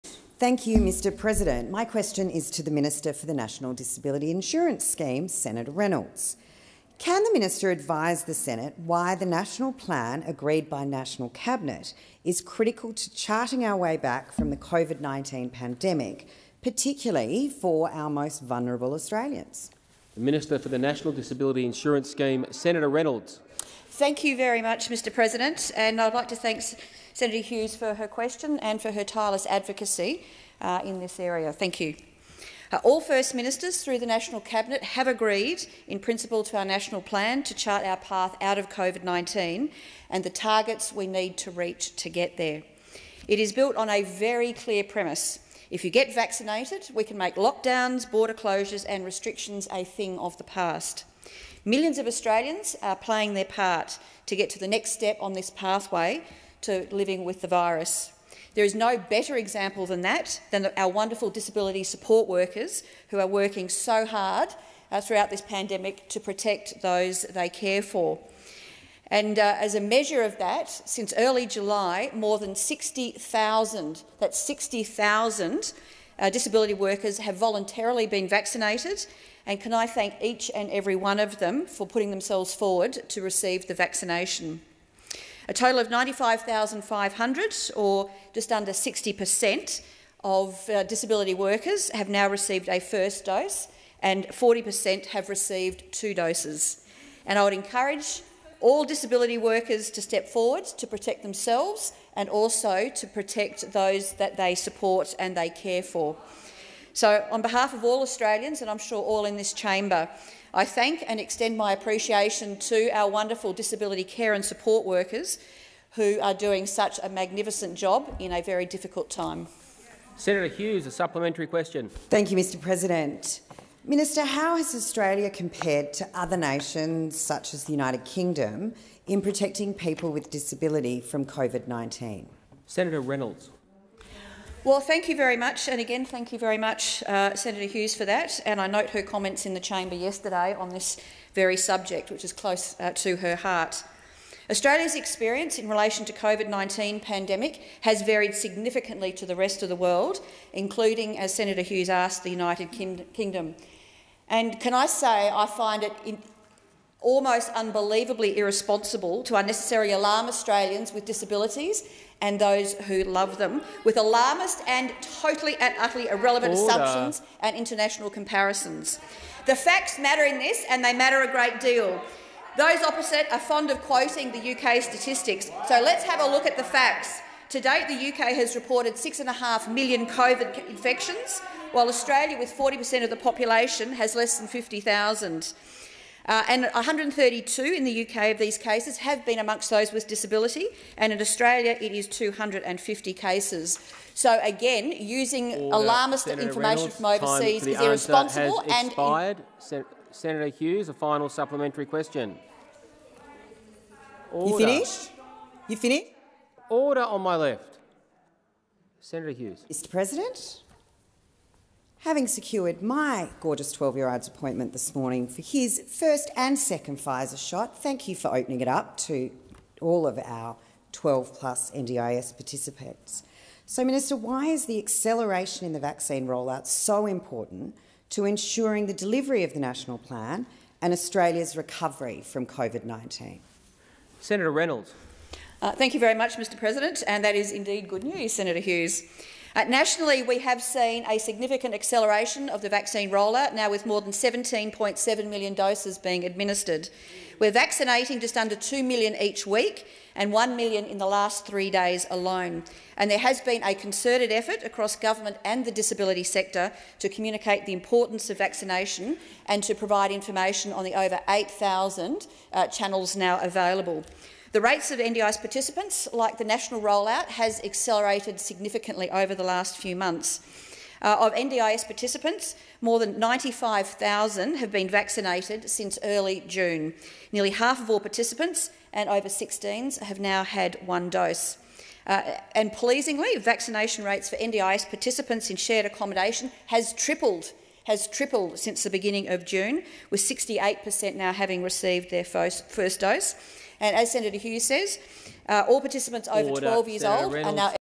The Morrison Governments' National Plan is crucial for vulnerable Australians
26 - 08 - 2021

Senator HUGHES:

My question is to the Minister for the National Disability Insurance Scheme, Senator Reynolds. Can the minister advise the Senate why the national plan, agreed by the national cabinet, is critical to charting our way back from the COVID-19 pandemic, particularly for our most vulnerable Australians?

Senator REYNOLDS:
I thank Senator Hughes for her question and for her tireless advocacy in this area. All first ministers through the national cabinet have agreed in principle to our national plan to chart our path out of COVID-19 and the targets we need to reach to get there. It is built on a very clear premise: if you get vaccinated, we can make lockdowns, border closures and restrictions a thing of the past. Millions of Australians are playing their part to get to the next step on this pathway to living with the virus.

There is no better example of that than our wonderful disability support workers, who are working so hard throughout this pandemic to protect those they care for. As a measure of that, since early July more than 60,000 disability workers have voluntarily been vaccinated, and I thank each and every one of them for putting themselves forward to receive the vaccination. A total of 95,500, or just under 60 per cent, disability workers have now received a first dose and 40 per cent have received two doses. I encourage all disability workers to step forward to protect themselves and to protect those they support and care for. On behalf of all Australians—and I'm sure all in this chamber—I thank and extend my appreciation to our wonderful disability care and support workers, who are doing such a magnificent job in a very difficult time.

Senator HUGHES:
Minister, how does Australia compare to other nations, such as the United Kingdom, in protecting people with disability from COVID-19?

Senator REYNOLDS:
Again I thank Senator Hughes. I note her comments in the chamber yesterday on this very subject, which is close to her heart. Australia's experience in the COVID-19 pandemic has varied significantly to the rest of the world, including the United Kingdom, which Senator Hughes asked about. I find it almost unbelievably irresponsible to unnecessarily alarm Australians with disabilities and those who love them with alarmist and totally and utterly irrelevant assumptions and international comparisons.

The facts matter in this, and they matter a great deal. Those opposite are fond of quoting the UK statistics, so let's have a look at the facts. To date the UK has reported 6½ million COVID infections while Australia, with 40 per cent of the population, has less than 50,000. In the UK 132 of these cases have been amongst those with disability, and in Australia it is 250 cases. Again, using alarmist information from overseas is irresponsible. (Time expired)

Senator HUGHES:
This morning I secured an appointment for my gorgeous 12-year-old's first and second Pfizer shots. Thank you for opening it up to all 12-year-old-plus NDIS participants. Minister, why is the acceleration in the vaccine rollout so important to ensuring the delivery of the national plan and Australia's recovery from COVID-19?

Senator REYNOLDS:
That is indeed good news, Senator Hughes. Nationally we have seen a significant acceleration of the vaccine rollout, with now more than 17.7 million doses administered. We are vaccinating just under two million each week—one million in the last three days alone. There has been a concerted effort across government and the disability sector to communicate the importance of vaccination and to provide information on the over 8,000 channels now available.

The rate of NDIS participants, like the national rollout, has accelerated significantly over the last few months. Since early June, more than 95,000 NDIS participants have been vaccinated. Nearly half of all participants over 16 years of age have now had one dose. Pleasingly, the vaccination rate for NDIS participants in shared accommodation has tripled since the beginning of June, with 68 per cent now having received their first dose. As Senator Hughes says, all participants over 12 years of age are now eligible for vaccination.